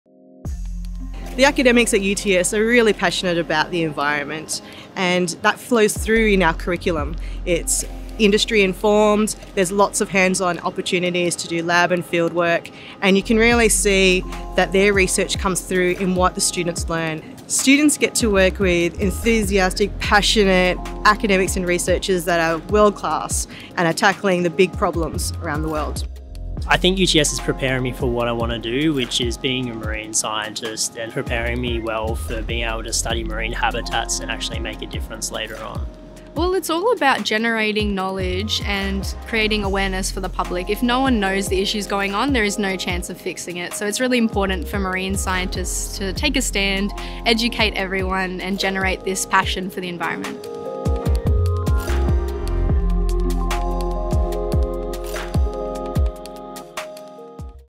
Why Study an Undergraduate Degree in Environmental & Marine Science at UTS.
Protect the species and ecosystems that hold the key to our planet’s survival.

Get your boots muddy — or your feet wet — with leading degrees in environmental and marine biology at UTS.

Our Environment and Marine courses combine general science subjects with specialist study in areas such as aquatic and terrestrial ecosystems, plant physiology, biodiversity conservation, wildlife management and environmental protection, among others.

These degrees will prepare you to work at the forefront of environmental protection and remediation where qualified scientists are more in demand than ever.